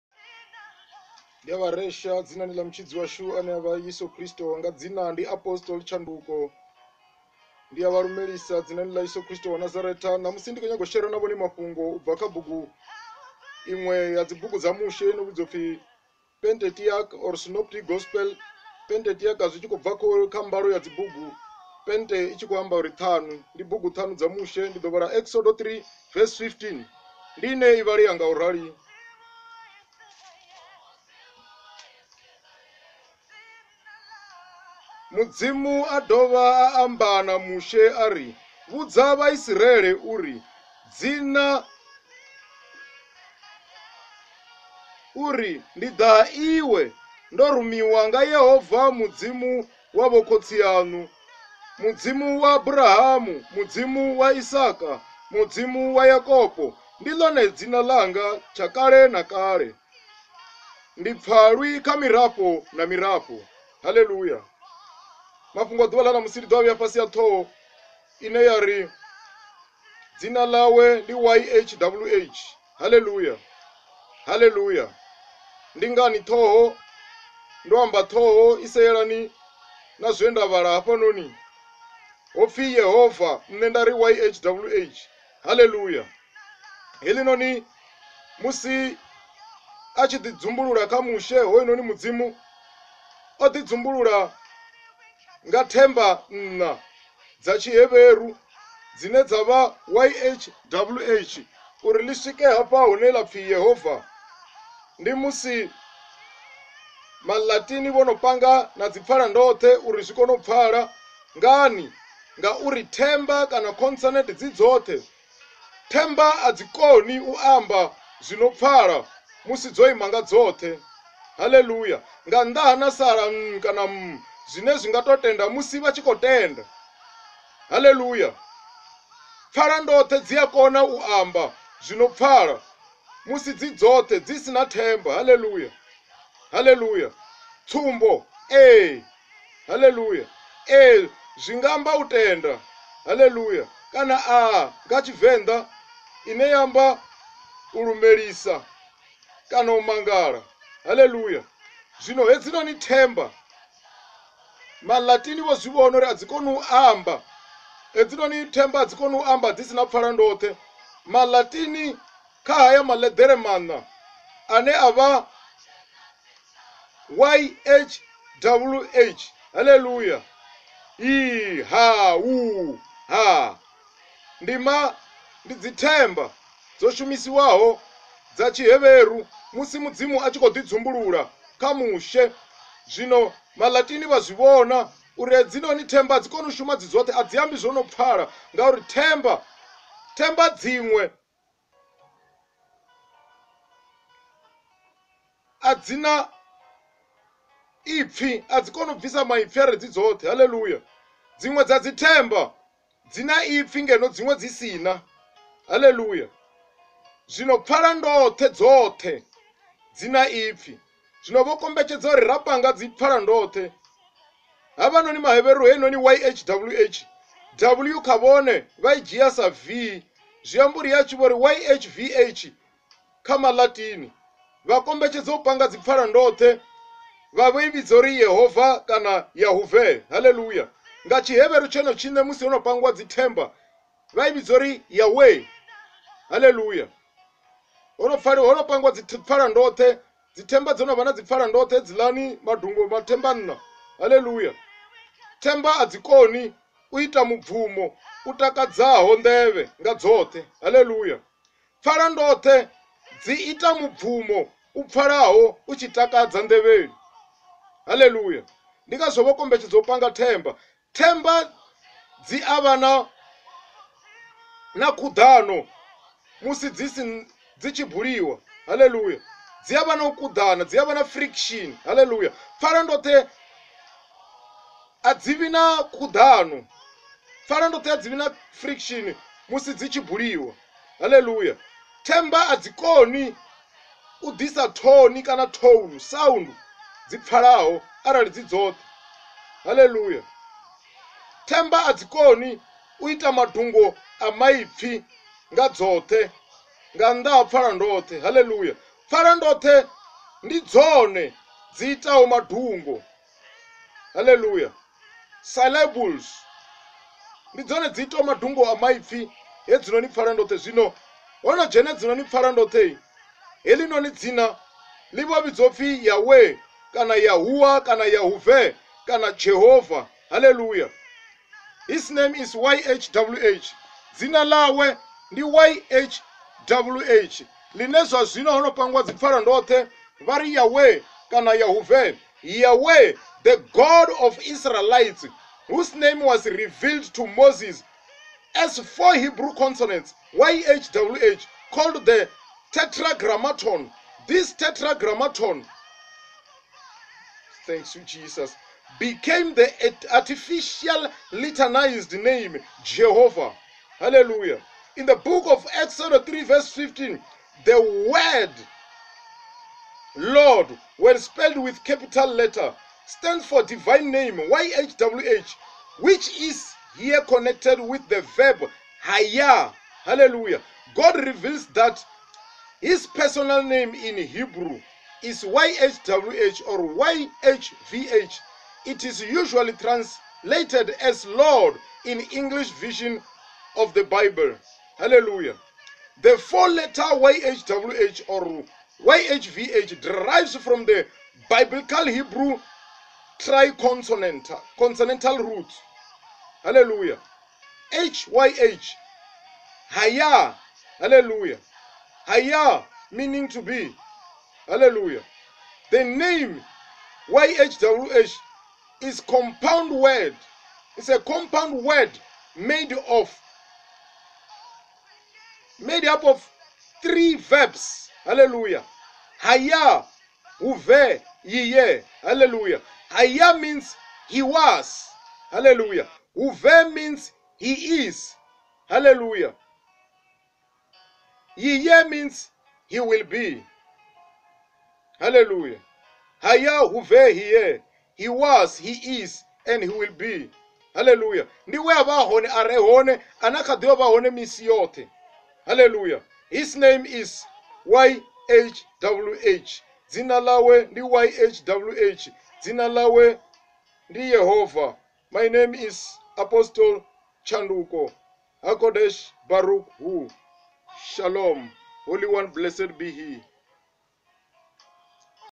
Dzina lawe ndi YHWH( His Name is YHWH)
God reveals that His personal Name in Hebrew is YHWH or YHVH. It is usually translated as LORD in the English version of the Bible. The four letter YHWH or YHVH derive from the Biblical Hebrew tri-consonantal root/verb h-y-h( hayah) meaning "to be".
The Name YHWH is a compound word made up of three verbs " hayah hoveh yi'yeh", meaning " He was, He is, He will be".